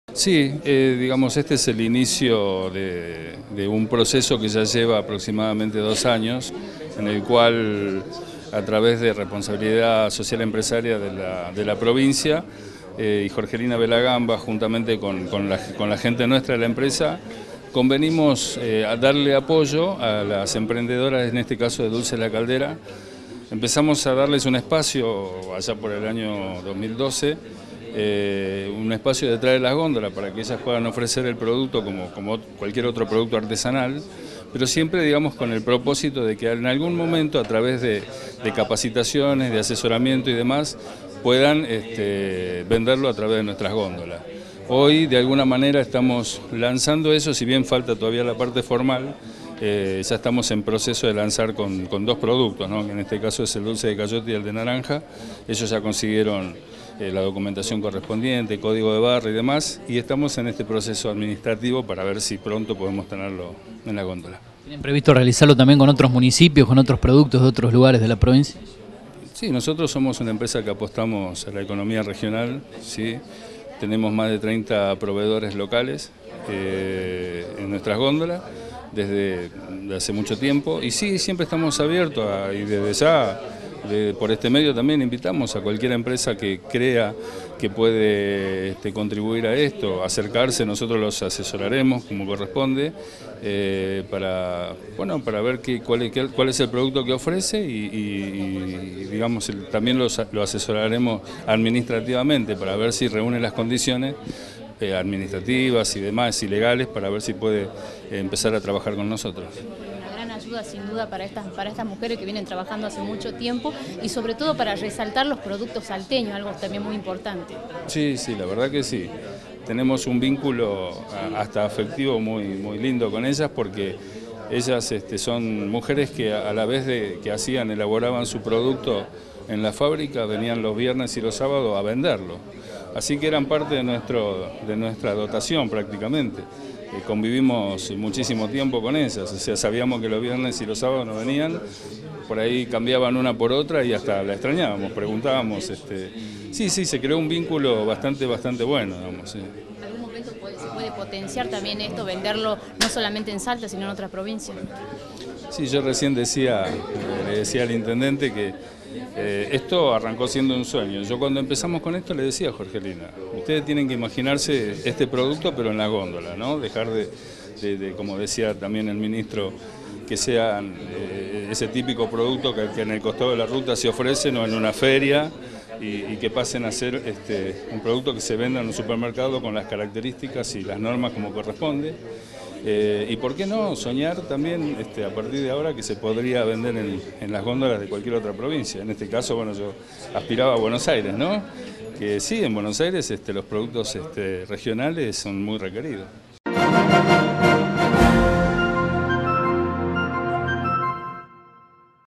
CARREFOUR INCLUIRÁ PRODUCTOS DULCES Y CONSERVAS-TV DOS SALTA
Julio Villalba, Gerente de Carrefour Salta. (DOS EN LA NOTICIA)